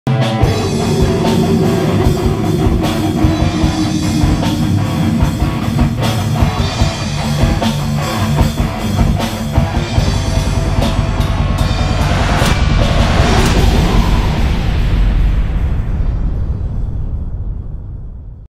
Blemias Flyer animado. Fecha junto a Destroyer 666
El jueves 12 de Mayo vamos a estar teloneando a la legendaria banda Destroyer 666, no se lo pueden perder.